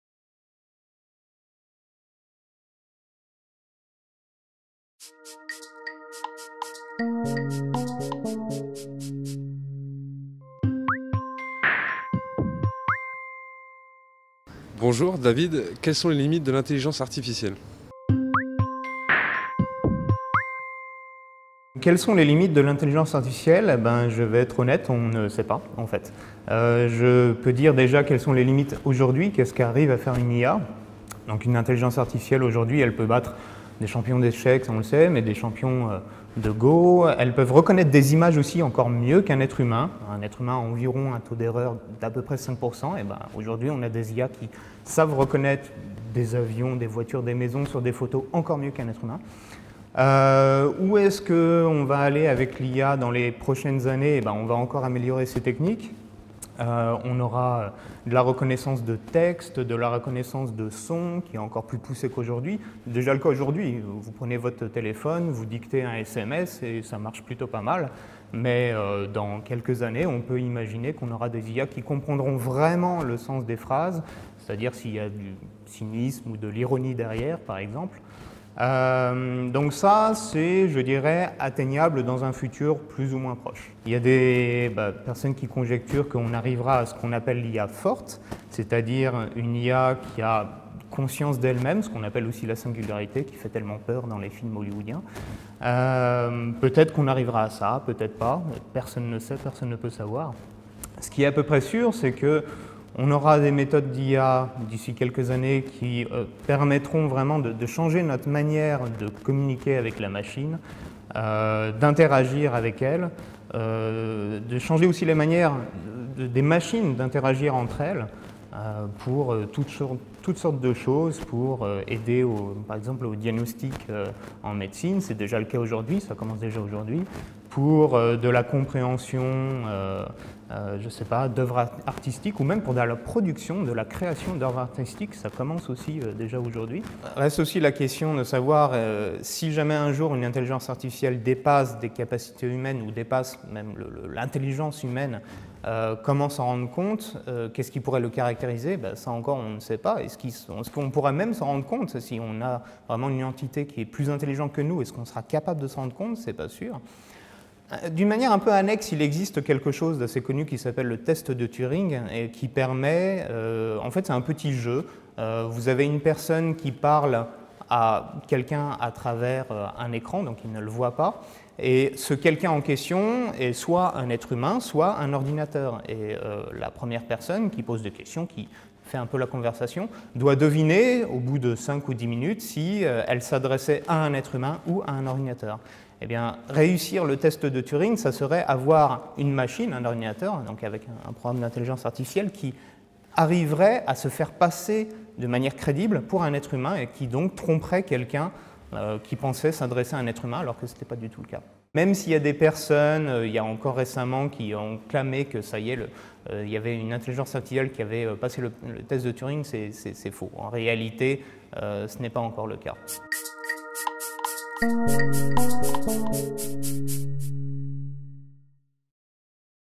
Quelles sont les limites de l'intelligence artificielle ?
L'intelligence artificielle a-t-elle des limites ? Que peut-elle déjà faire aujourd'hui ? Pourra t-elle un jour dépasser l'intelligence humaine ? Cette semaine, Florian Richoux, enseignant-chercheur à l'Université de Nantes, nous répond.